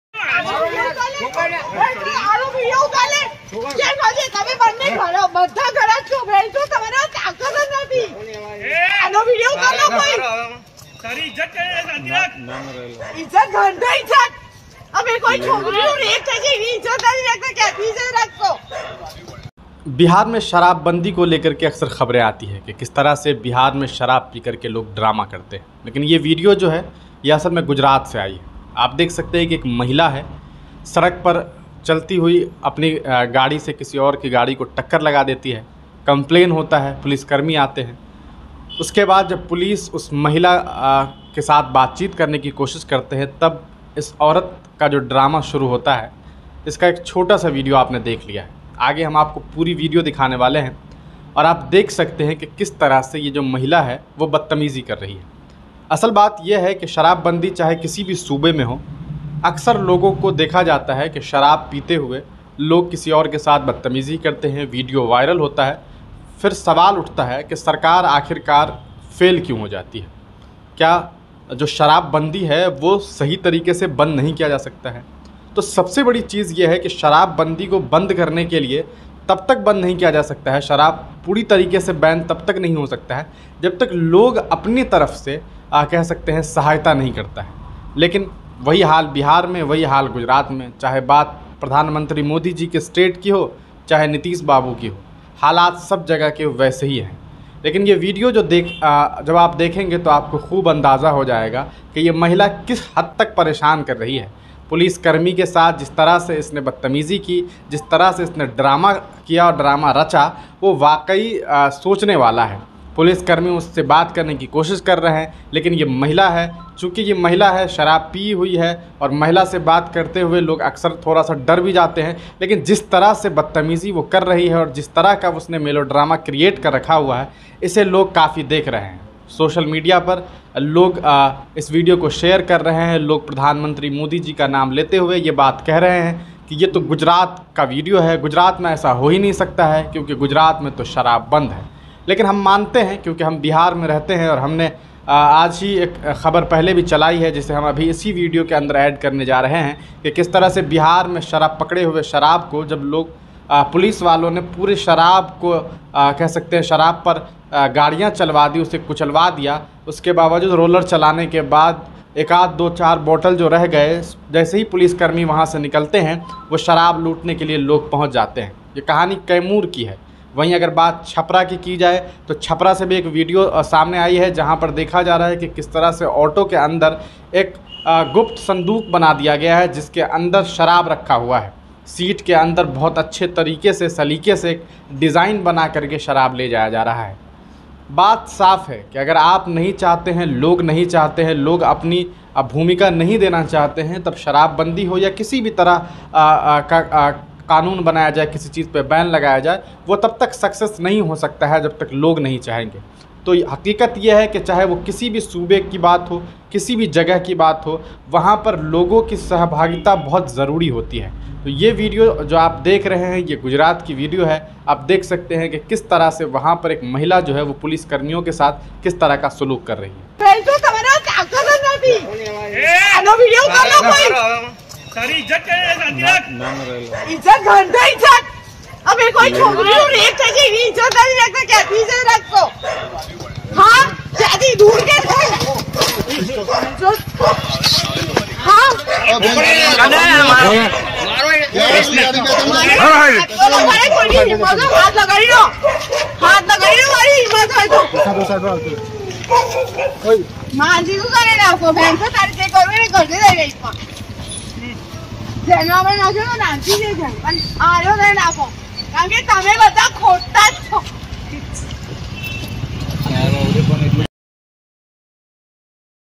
शराब की नशे में धुत लड़की की बदमाशी देखिये.. Gujrat के Vadodara में Police से उलझ गए, खूब हल्ला काटा
शराब के नशे में महिला का बीच सड़क हंगामा... पहले मारी गाड़ी को टक्कर फिर पुलिसवालों से की बदतमीजी!
गुजरात के वडोदरा की घटना.